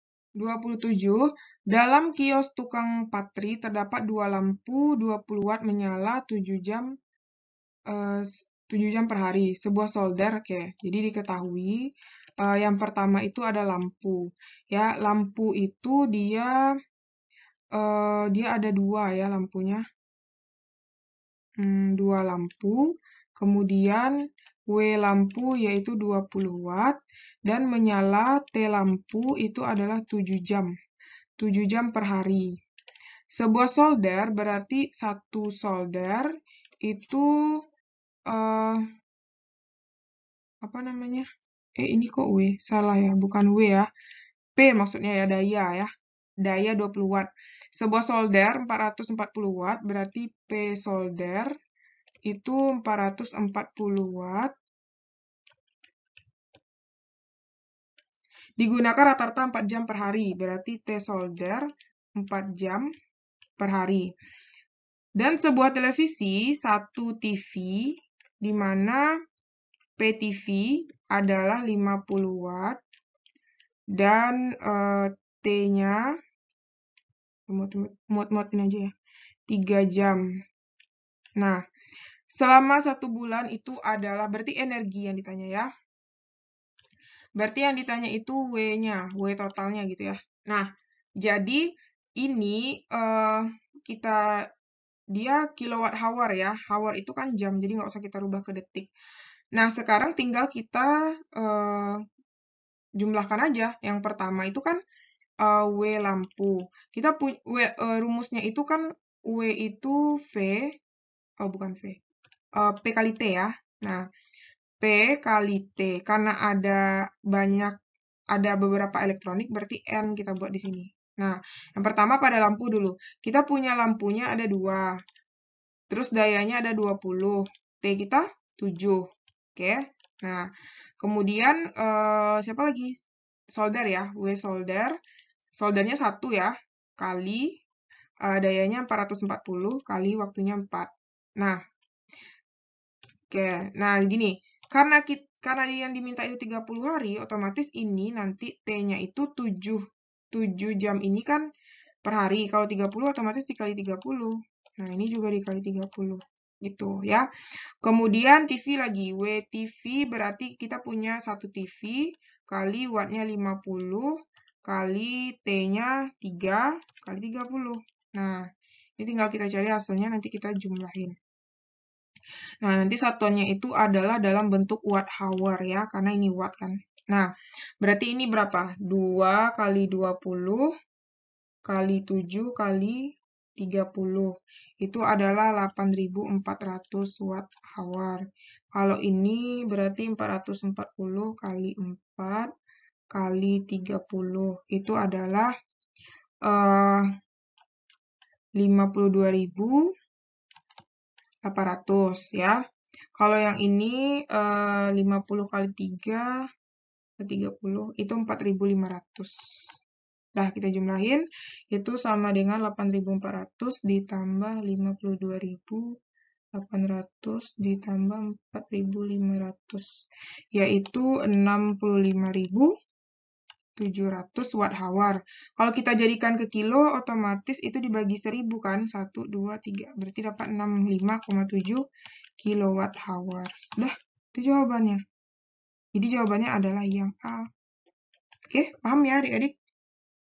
UK LISTRIK DINAMIS no 27 energi yang digunakan selama 30 hari
dalam kios tukang patri terdapat 2 lampu 20 qatt menyala 7 jam perhari, sebuah solder 440 watt digunkana rata-rata 4 jam perhari dan sebuah televisi 50 watt menyala 3 jam per hari. besarnya energi listrik yang digunakan selama satu bulan (30hari) adalah